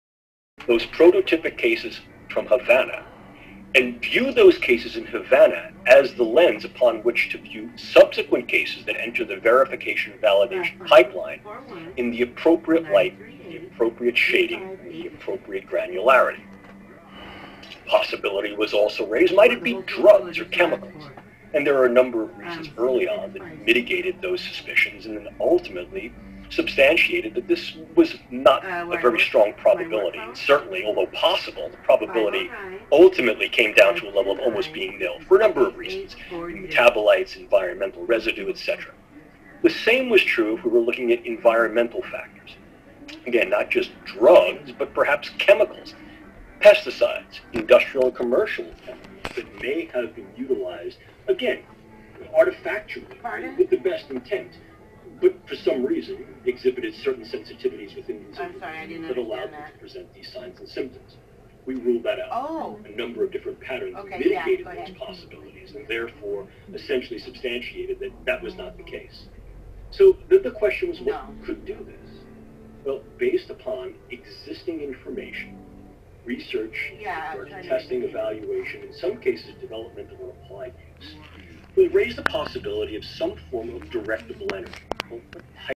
REC002 used as evidence to show the truth
As listening to the video you will hear them speak and sing to me these are Evil Gangstalkers that FBI needs to actually get up off their Butts and arrest someone!